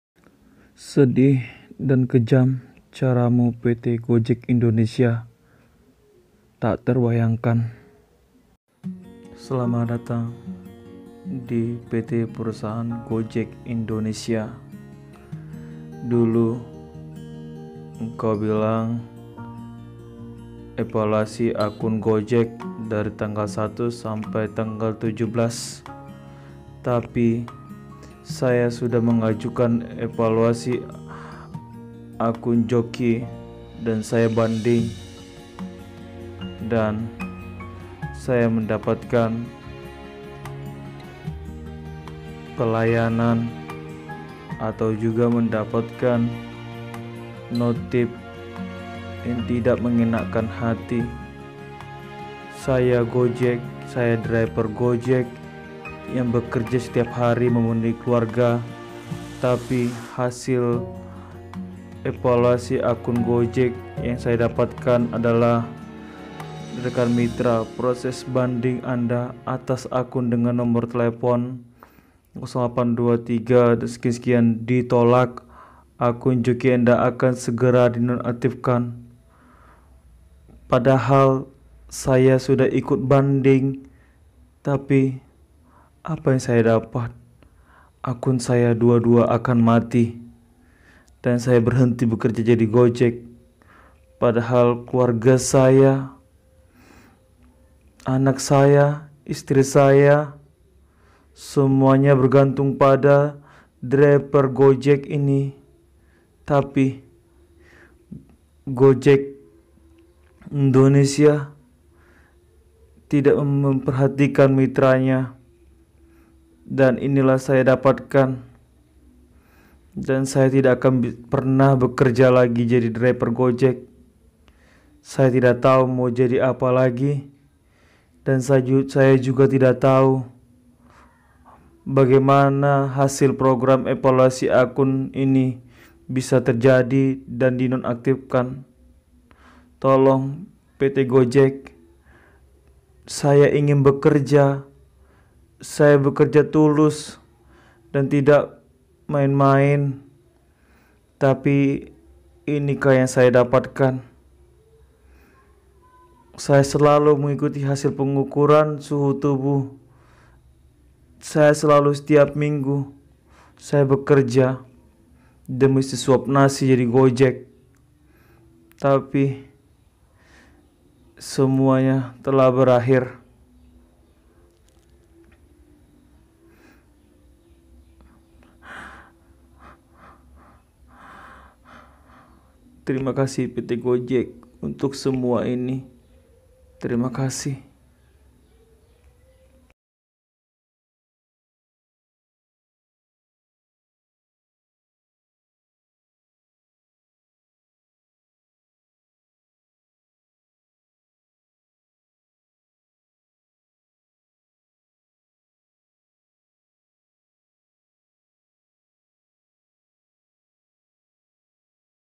Program evaluasi akun joki Gojek driver.akun joki dinonaktifkan akun pribadi tidak terbuka.sedihhh!!
Ini kisah nyata ..Sedih!!!evaluasi akun joki yg gagal paham.akun joki dinonaktifkan akun pribadi tidak terbuka.berani kan diri untuk banding supaya akun pribadi terbuka dan akun joki dinonaktifkan,ternyata malah akun pribadi tidak terbuka,akun joki di nonaktifkan,padahal saya bekerja sebagai driver Gojek sudah kerja menetap,bukan sampingan.akun pribadi pun di PM (putus mitra)diakibat kan karena sistem,bukan karena saya sengaja.dan saya tidak tau karena apa di PM.belom pernah Suspen 3 hari dan 7 hari,langsung putus mitra.alasannya cukup simple yaitu sistem mengatakan Ter INDIKASI MENJALANKAN ORDERAN FIKTIF, seandainya saya tahu itu orderan fiktif,saya pasti mengelak,tapi orderan mana itu,saya tidak tau.langsung di PM.dan saya banding,akhirnya di non aktifkan juga,,saya bekerja full di Gojek driver.program evaluasi akun joki Gojek driver.